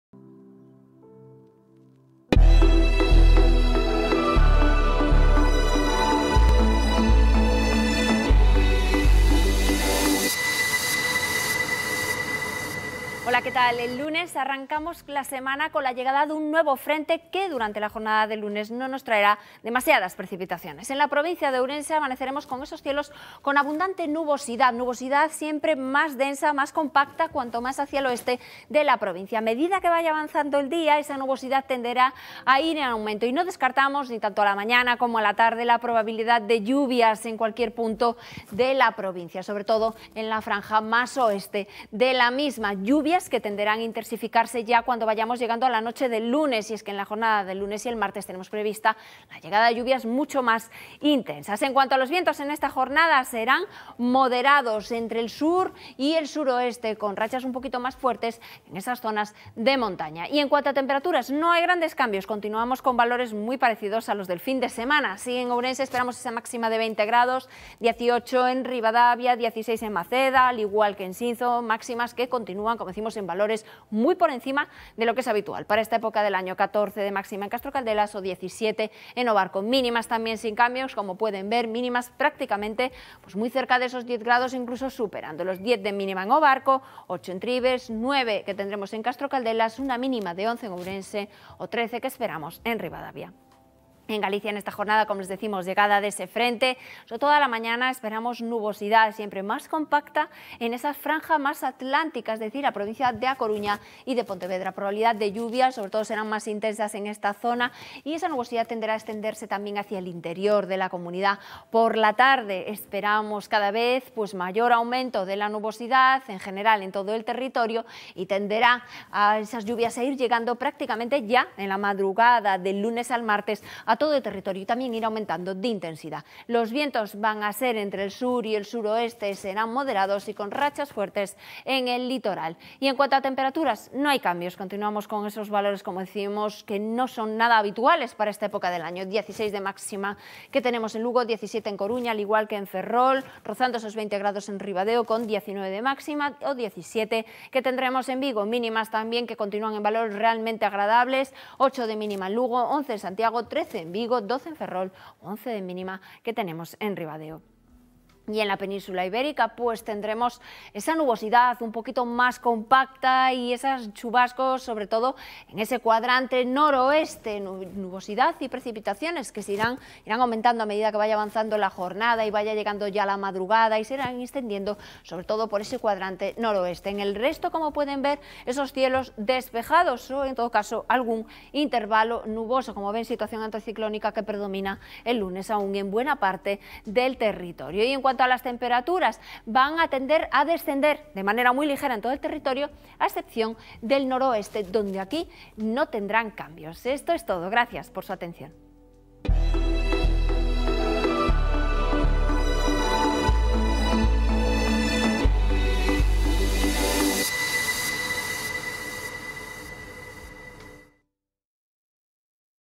¿Qué tiempo hará en Ourense este lunes?
Consulta la previsión meteorológica para la provincia este lunes 3 de enero.